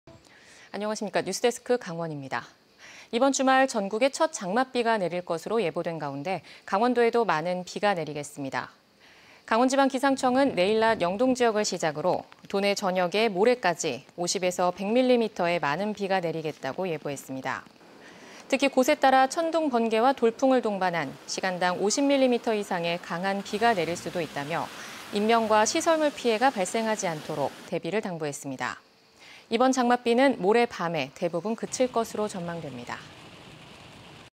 [단신] 주말 전국 장맛비, 강원도에도 많은 비 내려 210702
이번 주말 전국에 첫 장맛비가 내릴 것으로
예보된 가운데 강원도에도 많은 비가 내리겠습니다.
           ◀ＥＮＤ▶
 강원지방기상청은 내일(3) 낮 영동지역을
시작으로 도내 전역에 모레(4)까지 50~100mm의 많은 비가 내리겠다고 예보했습니다.

 특히 곳에 따라 천둥·번개와 돌풍을 동반한 시간당 50mm 이상의 강한 비가 내릴 수도 있겠다며, 인명과 시설물 피해가 발생하지 않도록 대비를 당부했습니다.

 이번 장맛비는 모레 밤에 대부분 그칠 것으로 전망됩니다.